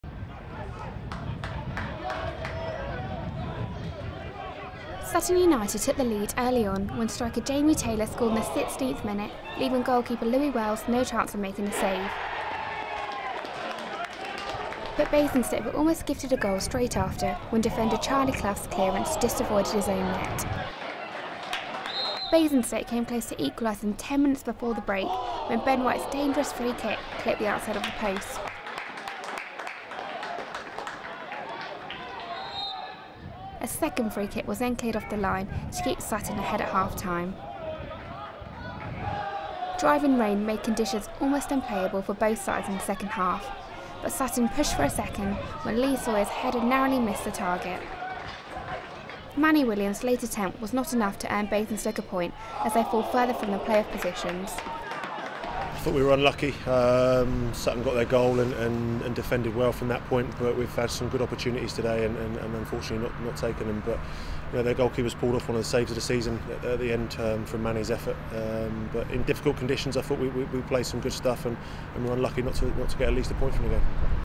Basingstoke Town vs Sutton United
Highlights of Basingstoke Town vs Sutton United in the Conference South